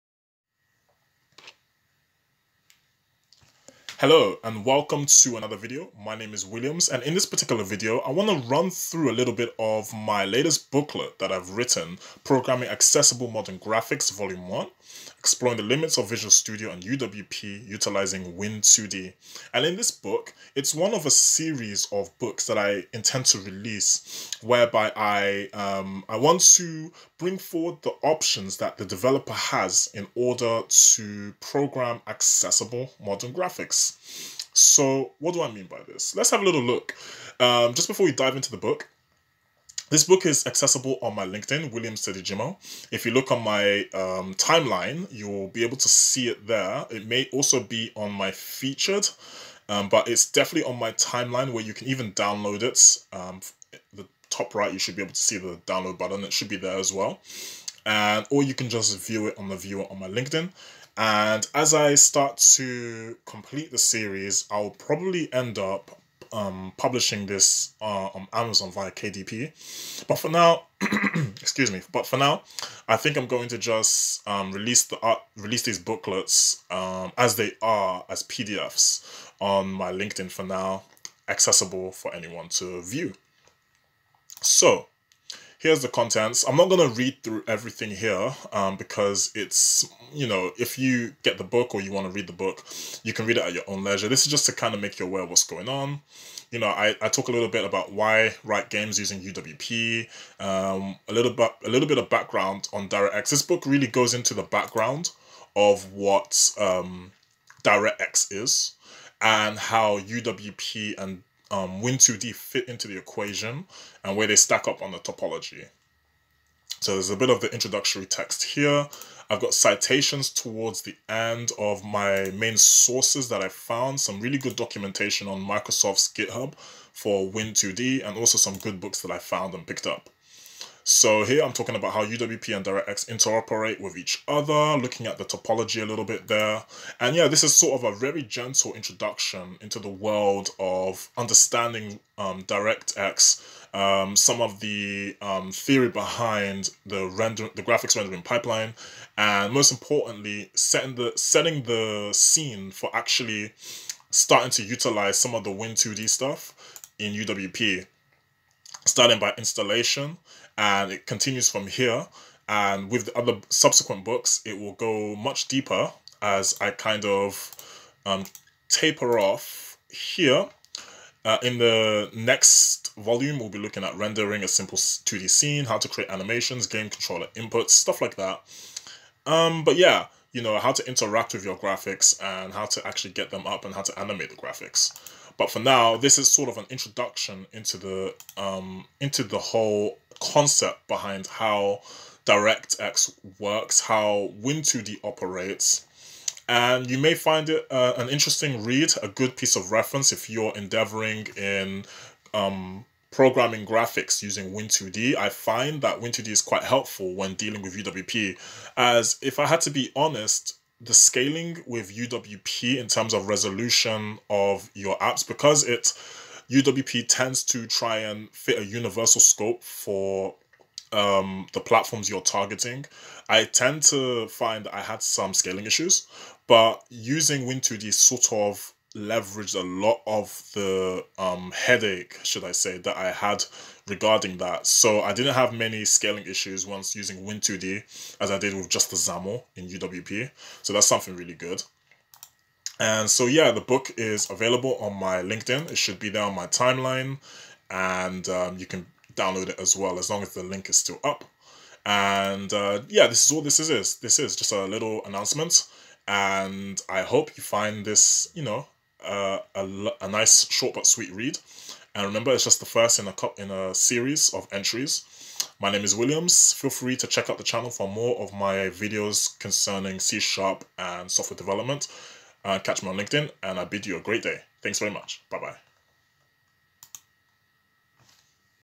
Direct X & Win2D Guide Booklet | Free PDF Available
Hi all, Will here. In my latest documentation I introduce the basics surrounding Direct X and Win2D for creating modern, accessible graphics-
a great primer for the programmer looking to introduce themselves to the concepts behind programming 2D graphics on a modern platform that provides accessible abstractions of powerful APIs.

Free PDF is available on my LinkedIn (On the Featured Section):
Williams Teddy-Jimoh